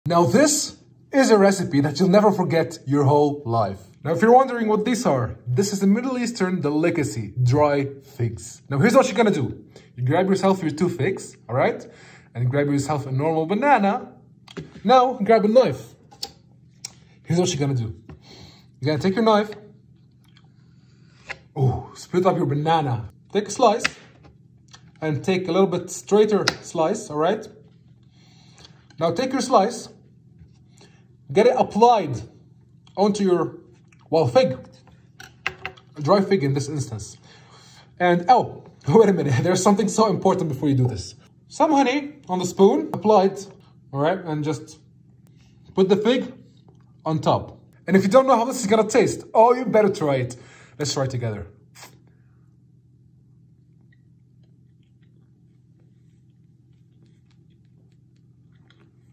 This SUPERFOOD Will Blow your MIND
Hey, foodie friends! Get ready for a fruity fiesta that'll make your taste buds do the cha-cha! 🍌🍯🎉 Join me in the kitchen as we whip up a recipe that's sweeter than your grandma's secret stash of candy. Picture this: ripe bananas, luscious figs, and a drizzle of honey that's so good, it'll have you licking the spoon like a kid in a candy store.

But wait, there's more! This isn't just any ordinary recipe - it's a flavor-packed adventure that'll have you feeling like a culinary wizard in no time. Think of it as your passport to flavor town, with fruity flavors that'll transport you to a tropical paradise (minus the pesky sunburn).

So, grab your apron and get ready to channel your inner chef as we dive headfirst into a whirlwind of fruity fun. Whether you're a seasoned pro or a kitchen newbie, this recipe is sure to be a hit with everyone from your picky-eater toddler to your Great Aunt Mildred. Let's get cooking, because when life gives you figs, bananas, and honey, you make magic happen! 🌈